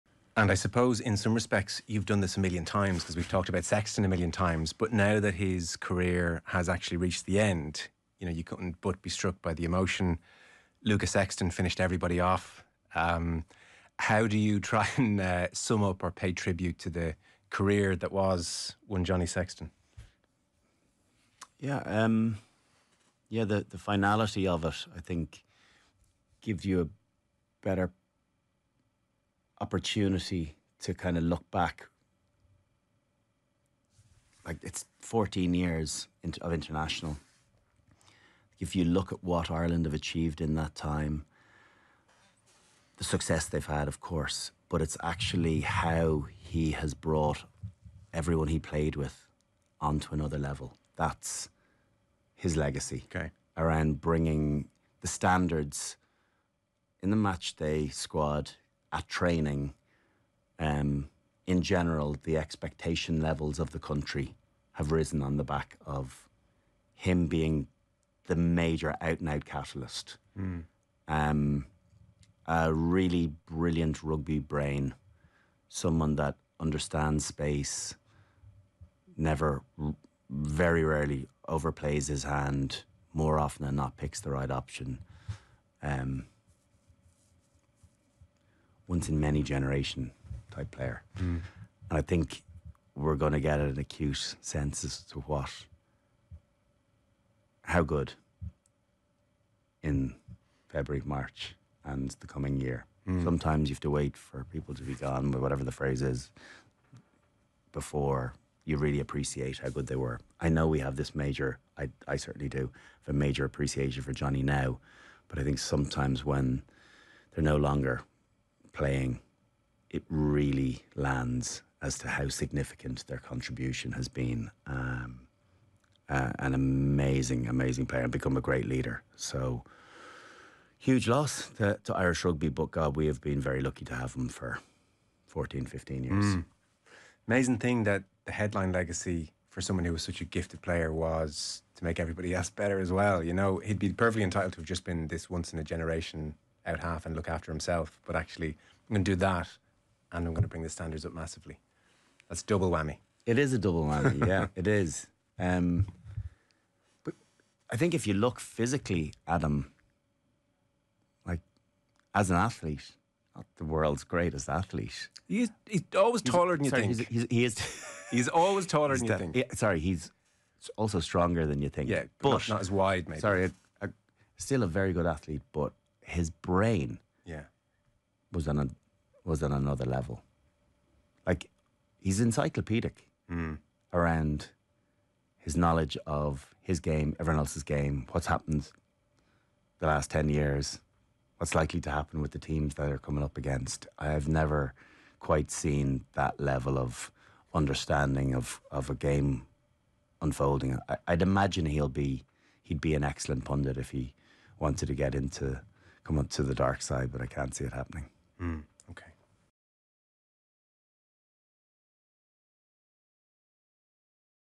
JOHNNY SEXTON: The legacy of an Irish rugby legend | BRIAN O'DRISCOLL
- Snapchat: add us and message the show @offtheball1